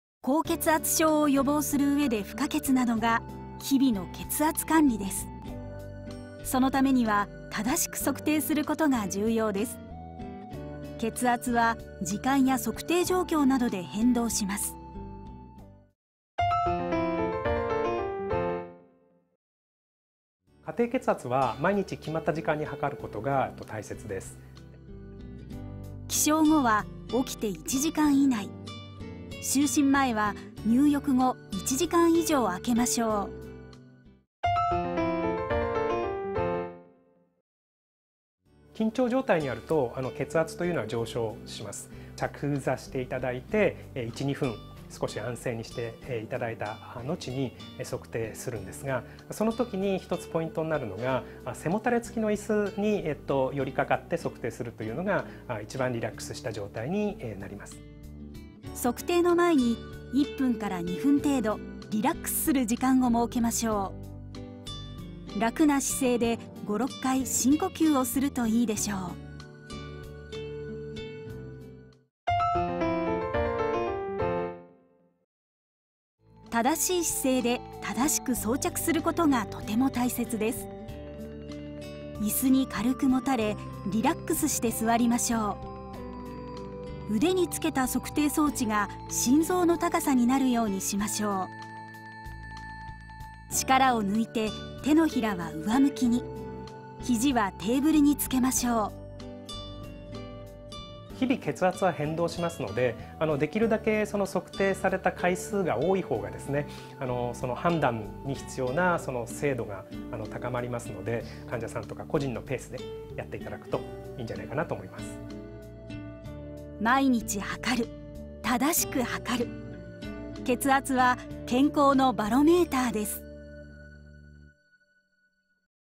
【正しく測ろう】血圧測定の方法とは【毎日測ろう】
KFBでは、「ウデをまくろう、ふくしま！」キャンペーンを展開しています。
血圧が気になる人も、そうでない人も。
血圧測定の習慣を作って、健康を管理しましょう。

血圧測定の方法を学び、正しい数値を計測しましょう。